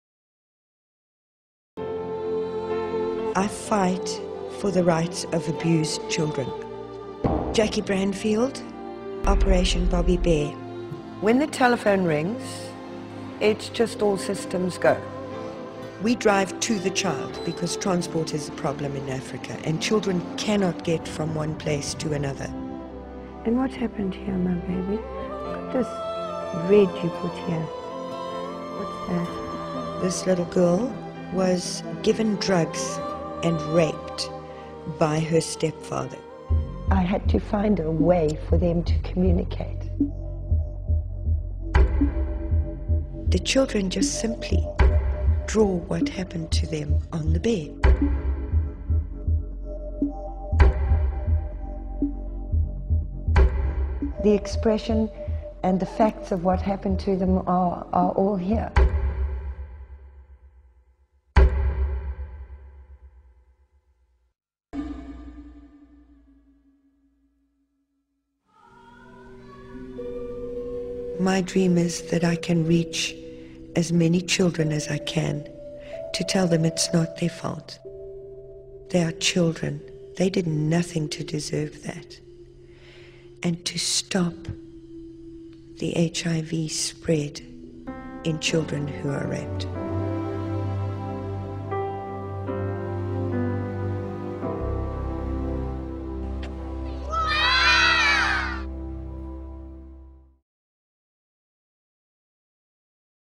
Life Ball 2012 - Crystal of Hope Awardee "Operation Bobbi Bear"
Since 2005 AIDS LIFE, the association behind Life Ball, has annually awarded an organization for outstanding projects in the fight against HIV/AIDS with the Crystal of Hope donated by Swarovski. In 2012 the award went to the South African project "Operation Bobbi Bear" founded by Jackie Branfield. The project fights for the rights of sexually abused children and tries to prevent them from HIV infections.